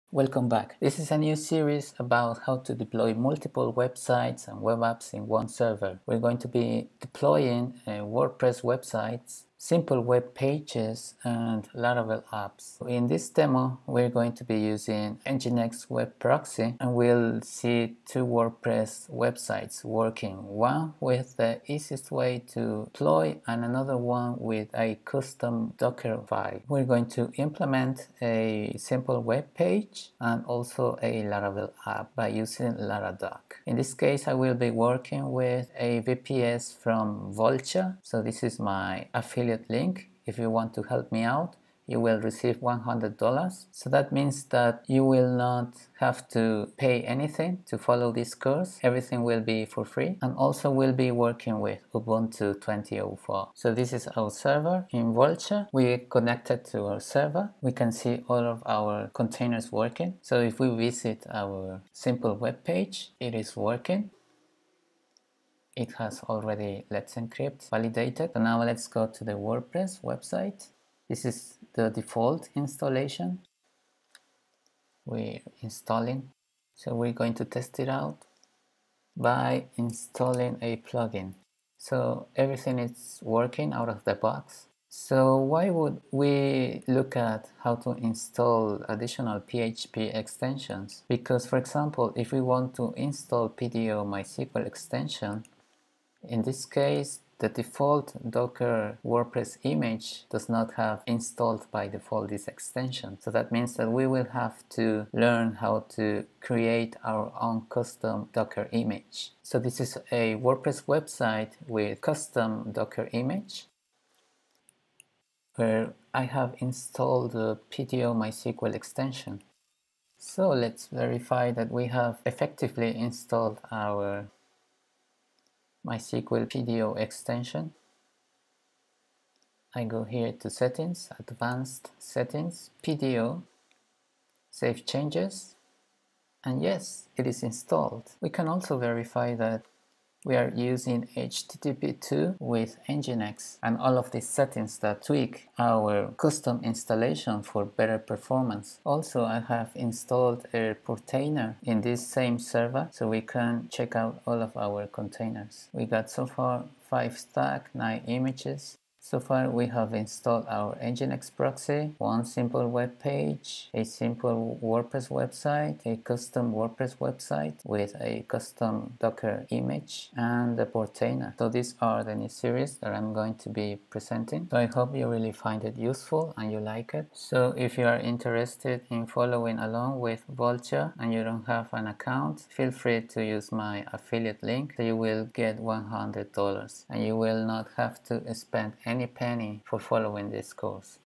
Deploy multiple WordPress and web apps in one server with Docker - Ubuntu edition - intro - 1
Deploy multiple WordPress websites and Laravel applications in one server with Docker - Ubuntu edition.

I am using Vultr. You can follow this course for free! and get $100 dollars.

How to deploy multiple WordPress websites, Laravel web applications, and simple HTML pages in one server by using Docker containers.
How to install Docker.
How to install an Ubuntu server in a VPS and secure it.
How to organize your web applications folders structure.
How to install and configure an Nginx web proxy.
How to set up your domains for each web application.
How to secure your websites with Let’s Encrypt HTTPS.
How to deploy more than one WordPress website, including a simple HTML-CSS webpage.
How to install and set up Laradock. A tool, used in this case, for Laravel Deployment.
How to deploy more than one Laravel application.
The most common commands of Linux, Docker, git, and Laravel deployment.

More to come:

In this video:
I present the introduction of what we'll learn in this newly updated series.